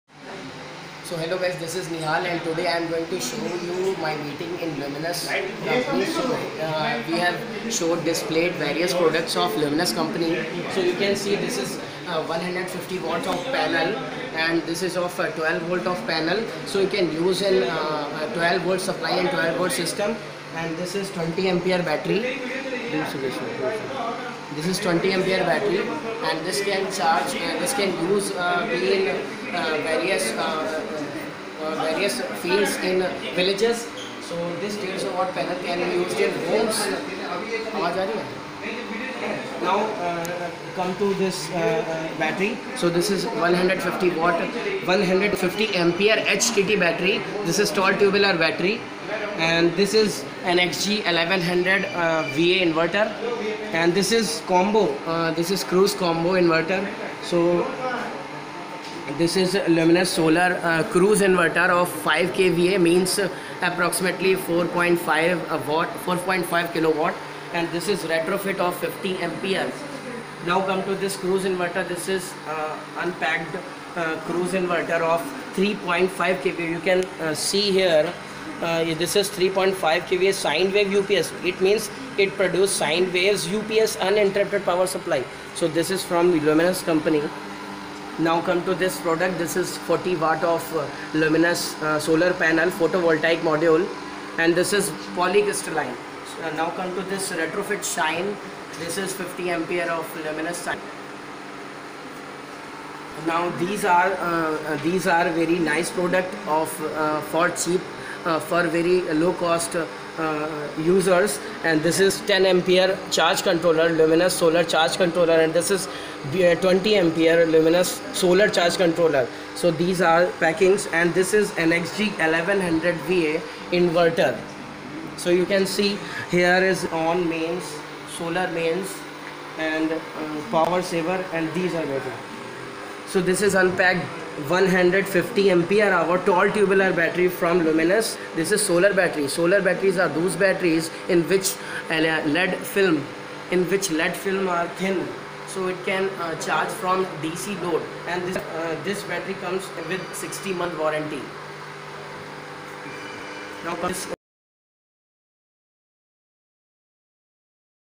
Luminous Solar Products showcase in Basant Leela Meeting
In this video I described various solar products from Luminous Company in Basant Leela Dealers Meet.